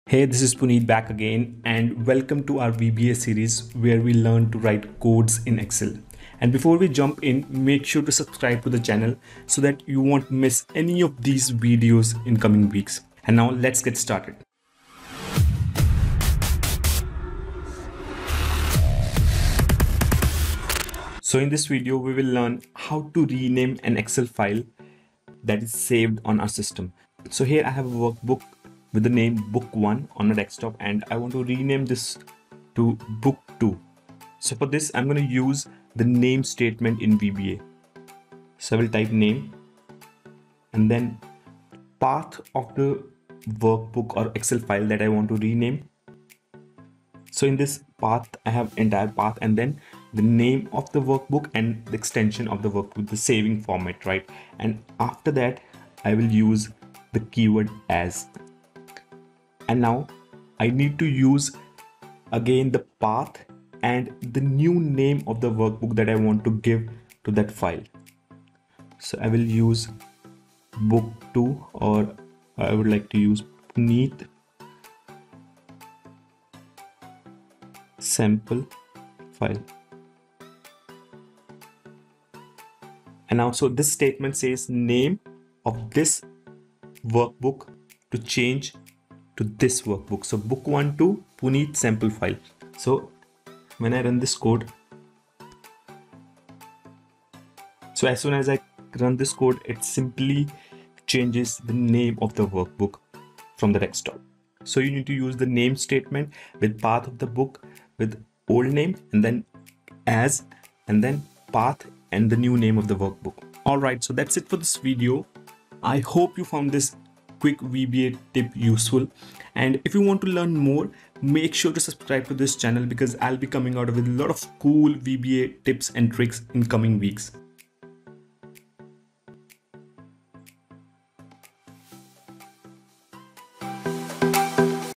Rename an Excel File (Workbook) using a Macro | Excel VBA
In this video, we will learn how to rename an Excel file that is saved on our system. I have a workbook with the name book1 on my desktop and I want to rename this to book two. For this, I'm going to use the NAME statement.

-Enter NAME and then path of the workbook or workbook including the name of the workbook.
-Then use the AS keyword and then path of the workbook including the new name that you want to give.

Here's the complete blog post, if you like to read